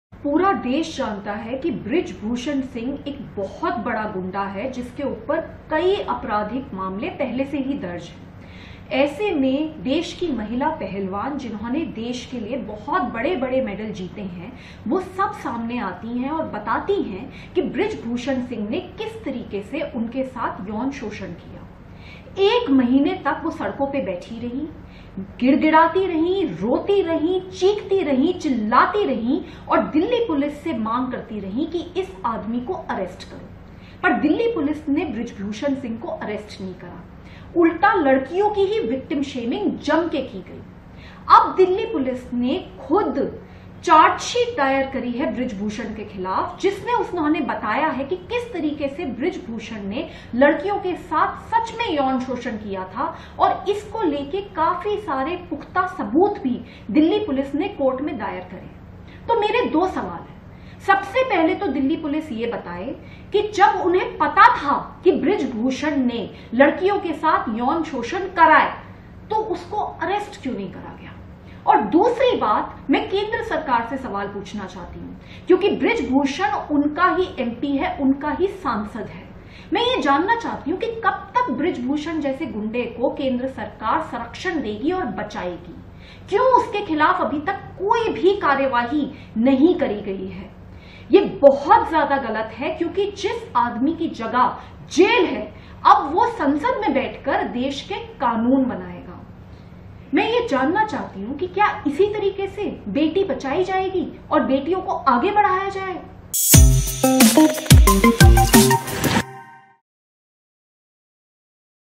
Brij Bhushan और पहलवानों वाले मुद्दे पर Delhi Police पर भड़कीं Swati Maliwal | Wrestlers protest
यौन उत्पीड़न के आरोपों का सामना कर रहे बीजेपी सांसद बृज भूषण शरण सिंह अब एक और वजह से चर्चाओं में आ गए हैं. उनका एक वीडियो सामने आया है, जिसमें वह महिला रिपोर्टर के साथ बदसलूकी करते नजर आ रहे हैं. इस वीडियो में वह रिपोर्टर पर गुस्सा करते और उसका माइक तोड़ते भी दिख रहे हैं. इस घटना को लेकर दिल्ली महिला आयोग की अध्यक्ष स्वाति मालीवाल की भी प्रतिक्रिया आई है और उन्होंने बृज भूषण शरण सिंह को गुंडा बताया है.